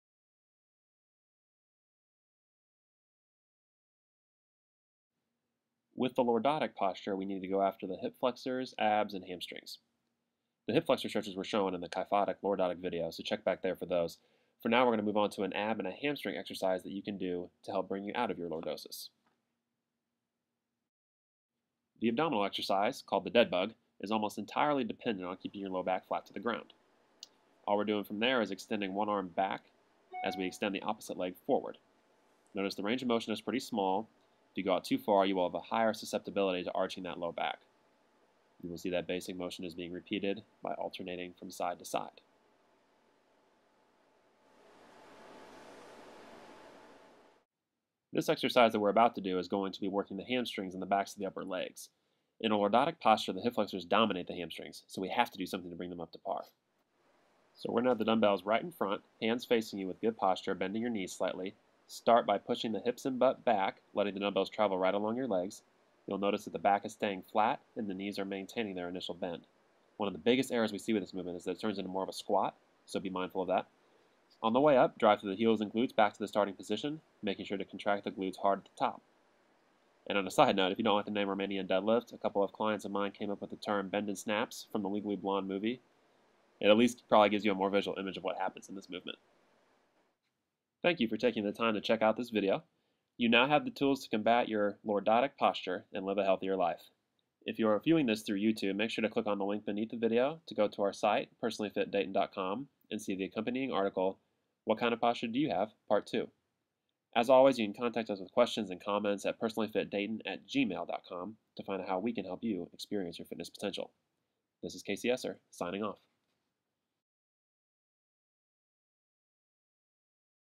Fix Your Posture: Lordotic
Imagine you feel sore, have a bad headache, can't breath very well, you're fatigued...oh and yeah...your back hurts.

Sound like you just had a car crash?

Now imagine you feel great, thinking clearly, and with lots of energy...pain-free.

Sound like a dream?

The first set of conditions are actually all potential, and common, consequences of having a poor posture!  I am going to guess that you are feeling or have felt at least 2 of those things in your lifetime.  The rest of you don't ever want to...just ask the first group; you probably know one or two of them.
The second set describes the wonders of what great posture can do for your body and mind.
Don't take poor posture personally.  It's not your fault.  After all, you have to make money for your family, and that, more often than not, requires sitting at a desk for 6-8 hours a day.  It is really easy to slip into bad habits hunched over a computer for such a prolonged period like that.
Some people simply have structural issues that have been present since birth, and greatly exacerbated by our sedentary society.  But whatever the case, you are not doomed to bad posture forever!

Excerpt from, "What Kind of Posture Do you Have?" (Part 1) by Kasey Esser, CSCS that can be seen